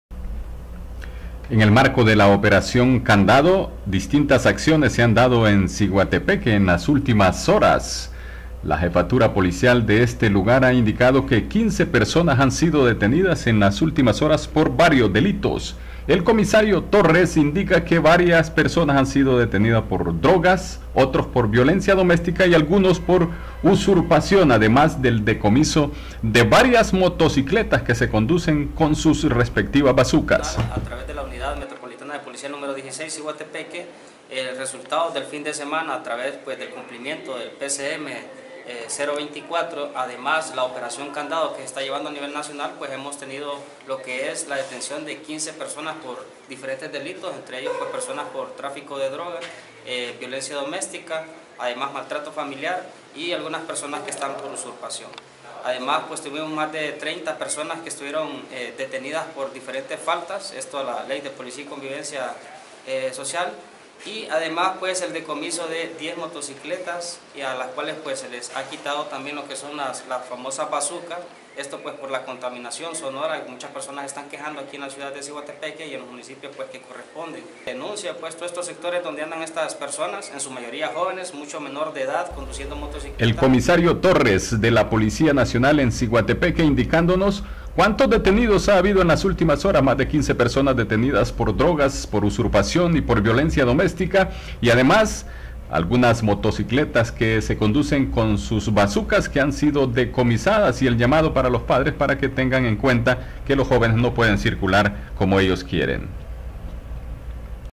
Operación candado deja 15 detenidos por drogas en Siguatepeque
El Comisario Torres relata las últimas detenciones en Siguatepeque,  muchos por trafico de drogas, y decomisos de motocicletas.